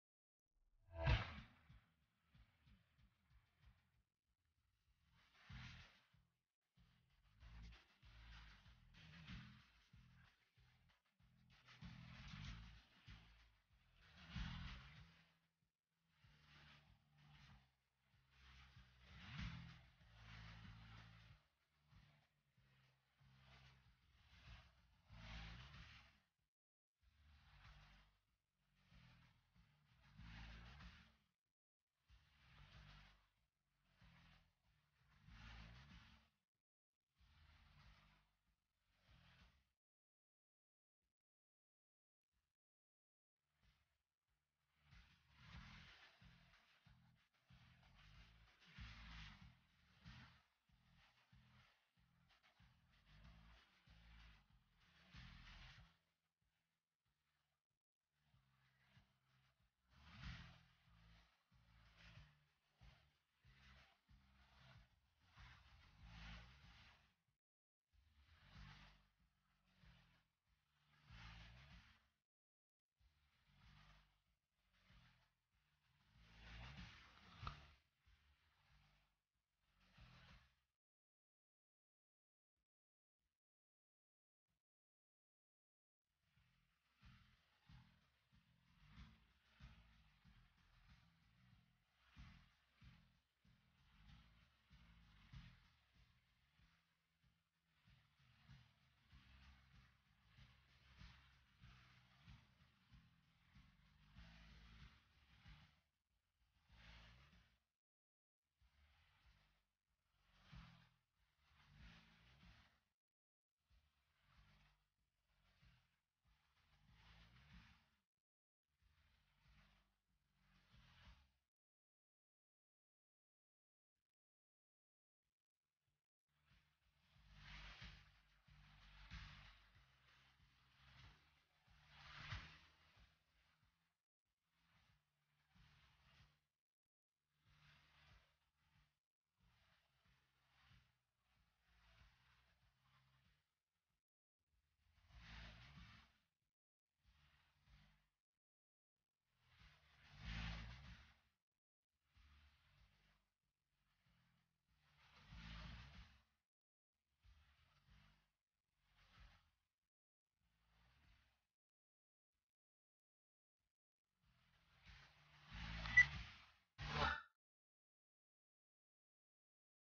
Have You Met King - At Grandpas - Playing Soccer.
1 year old King showing is skill to his grandpa.